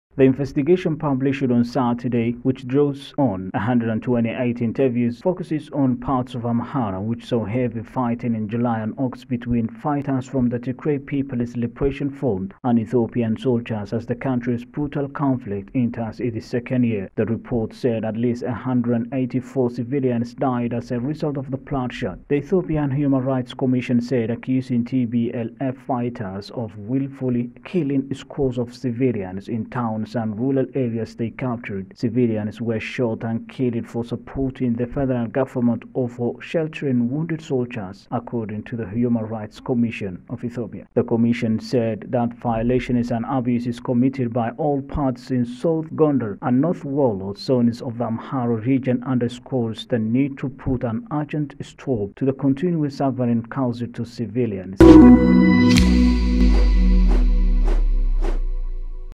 Ethiopia rights body accused TPLF for killing civilians
Tigrayan rebels have killed more than 150 civilians in the Amhara region on suspicion of being informants or offering aid to federal forces, the government-affiliated Ethiopian Human Rights Commission said in a report.